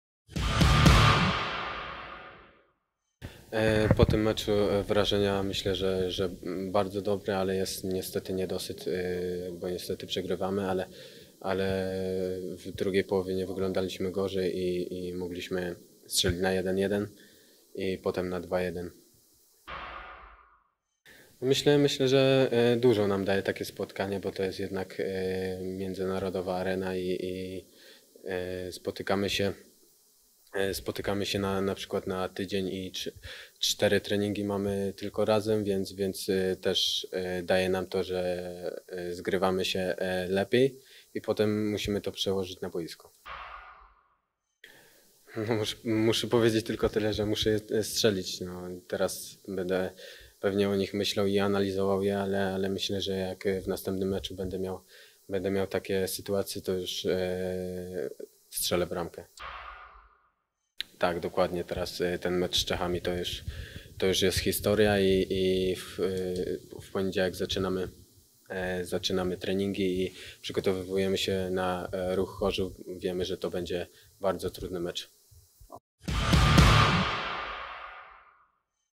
Fornalczyk: Pozostał niedosyt. Mogliśmy wygrać.
Pomocnik "Słoni" po meczu reprezentacji Polski U-20 z Czechami